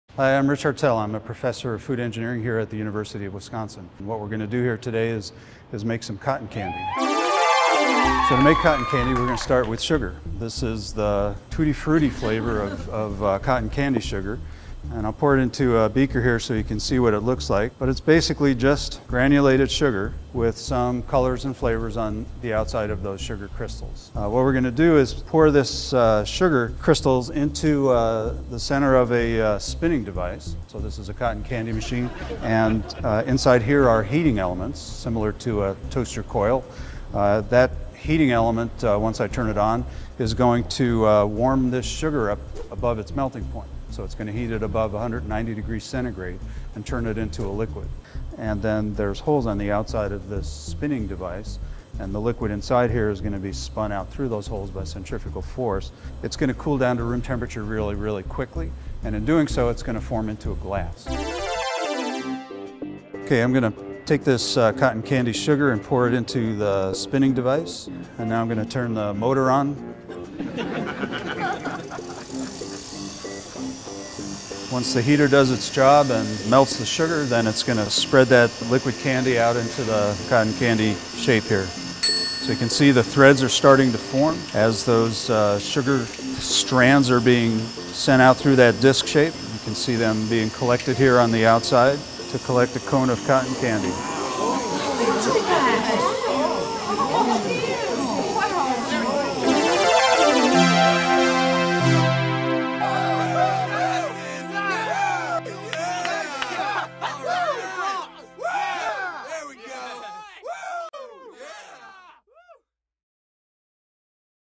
How to make cotton candy - Bytesize Science
So what is cotton candy? Watch as University of Wisconsin-Madison Professor of Food Engineering Richard Hartel explains how to spin piping-hot melted sugar into tasty threads, eventually cooling off and forming cotton candy.